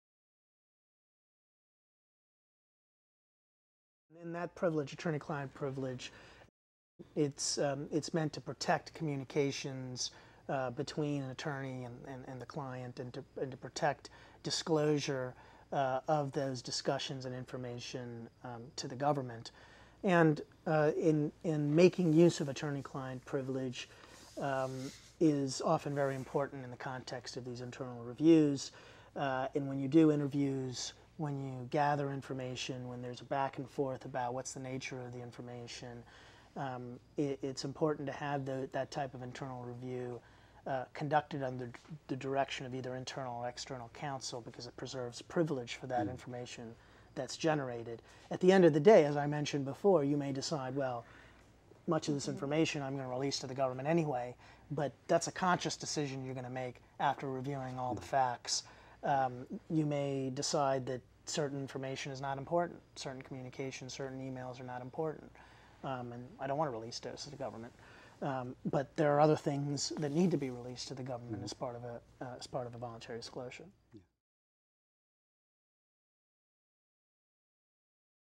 The Role of Attorney Client Privilege in the Voluntary Disclosure Process
Ajay Kuntamukkala, Partner, Hogan Lovells